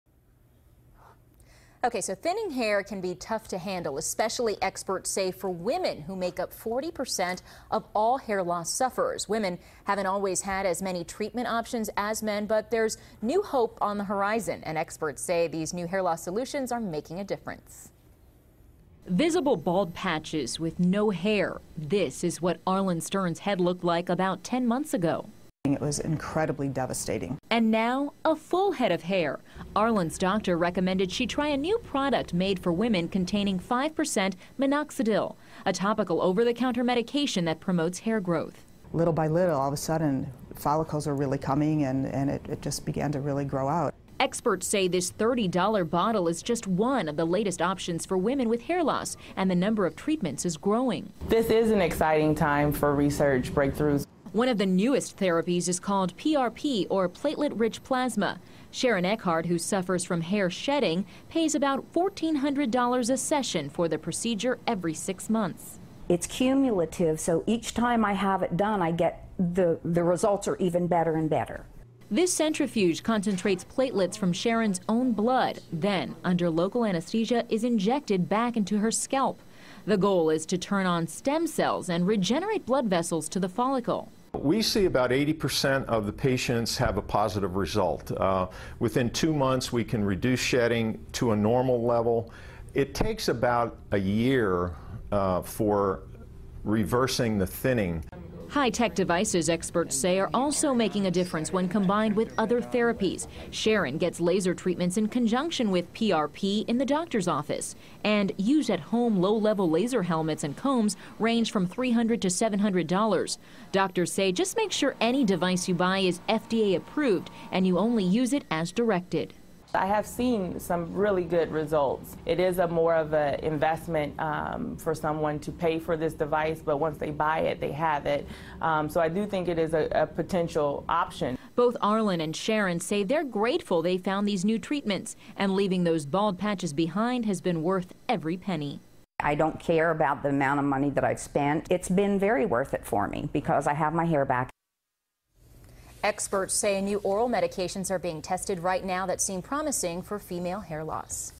New Hope For Female Hair Loss
Over the counter medication that promotes hair growth